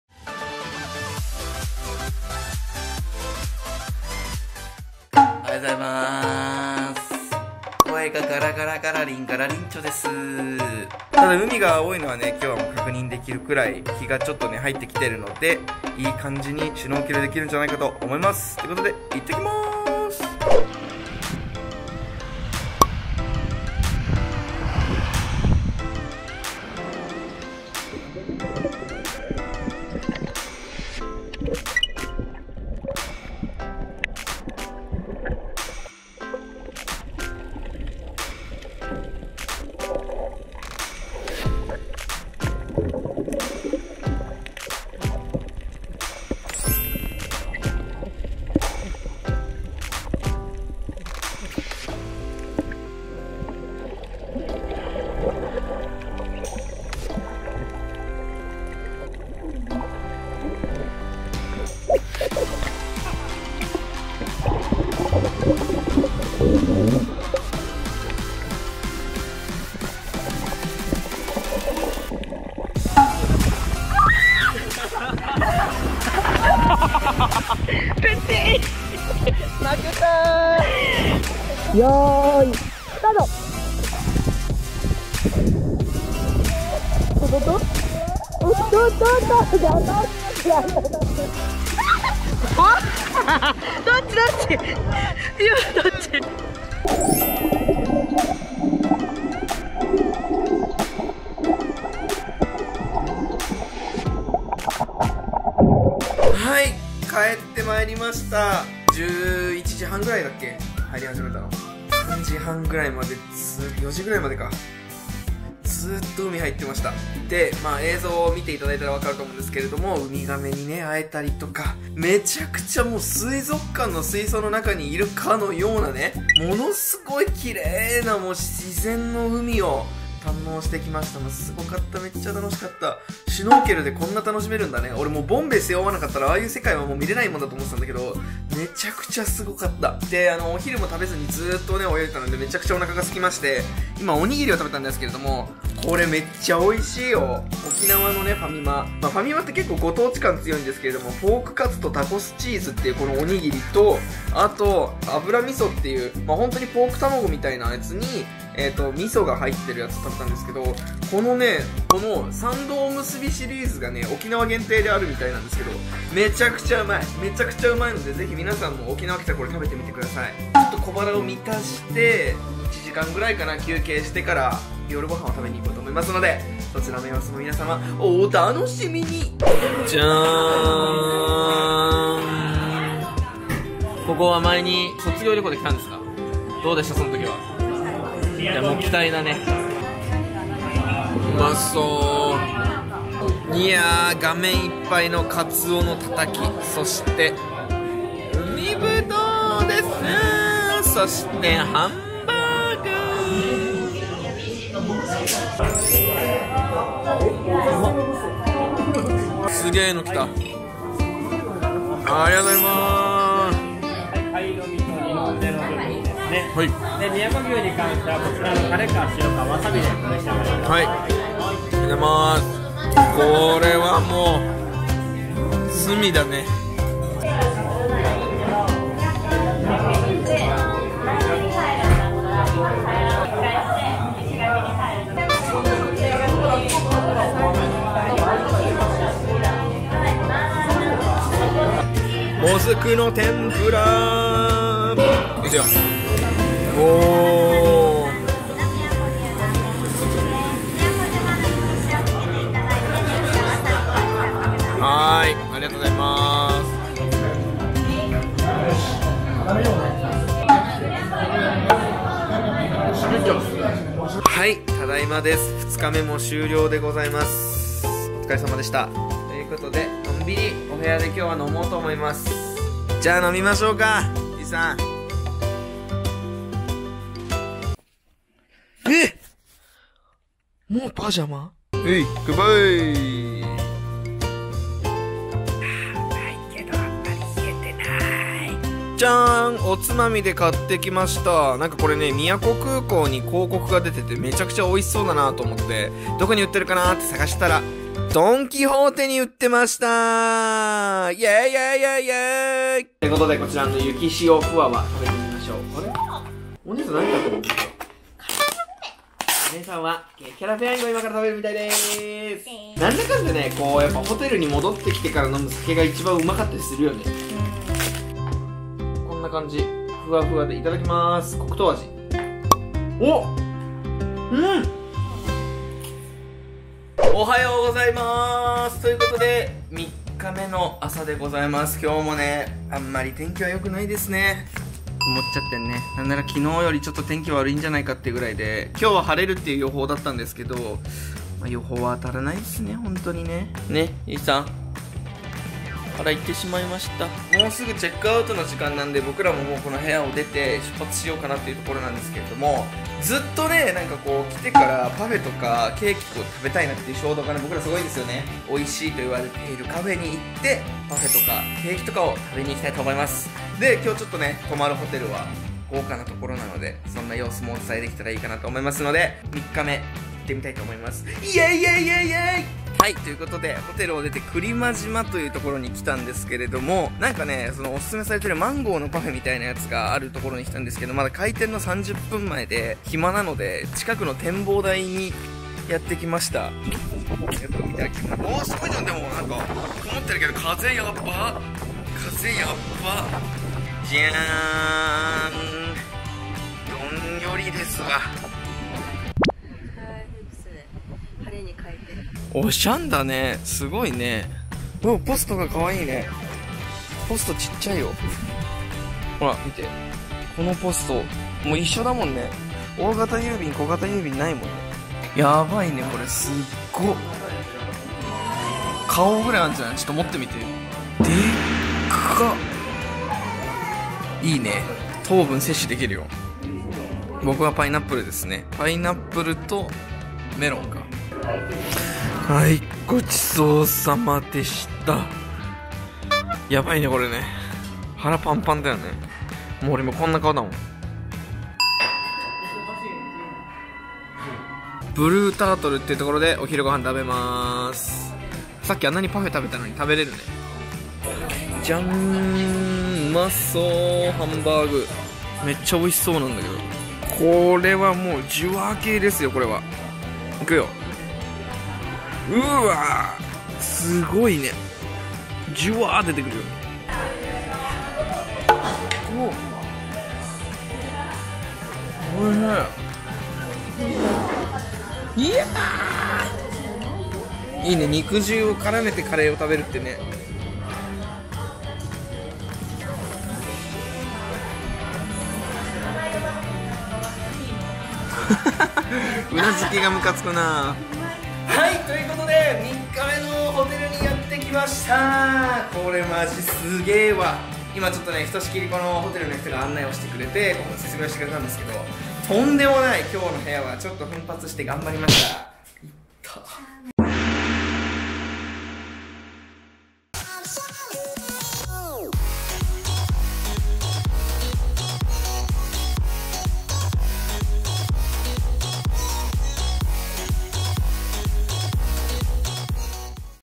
まさに天然の水族館！ウミガメの生息地でヤバいものに出会った！【さとゆい宮古島旅行2019】【Part2】
Part3
公開までお待ちを！

オープニング使用曲